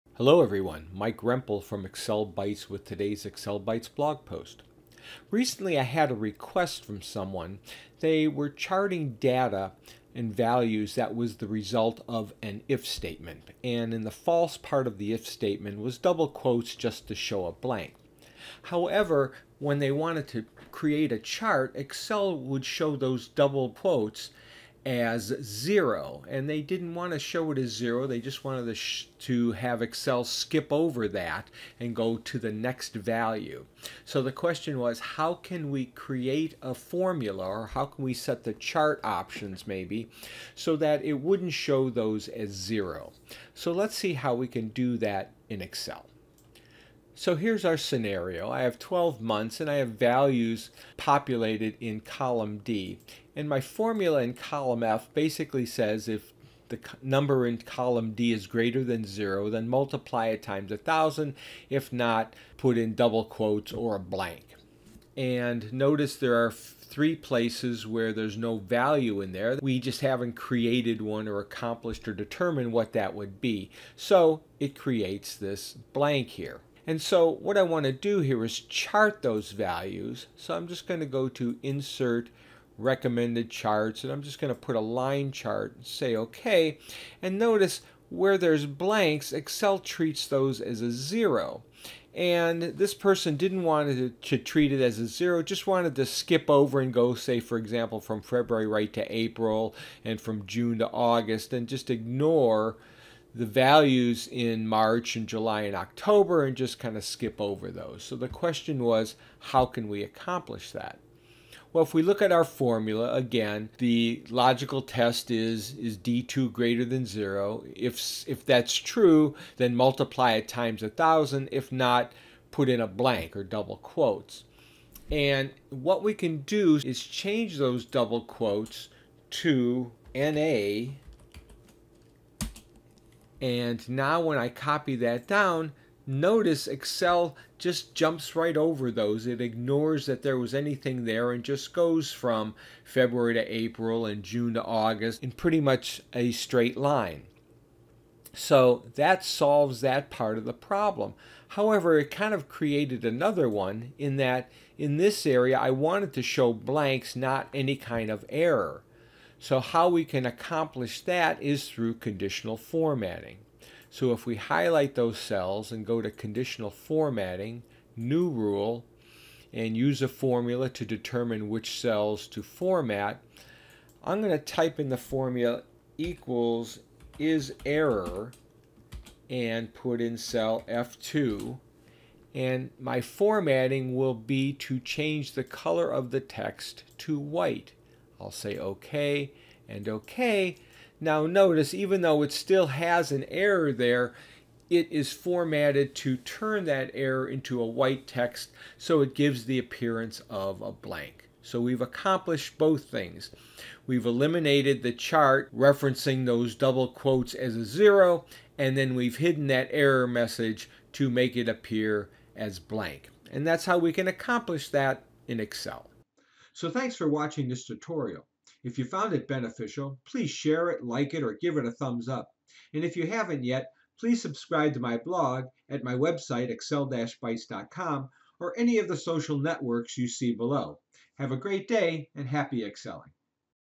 How To Show True Blanks In A Chart In Excel
I recently had a request to show TRUE blanks in a chart in Excel, when using an IF statement to determine the values, and the FALSE result of the logical statement is a blank, using double quotes. Excel shows that blank as a zero and the user wanted it just to be ignored and skip to the next value in the list. So let's see how we can do that in Excel.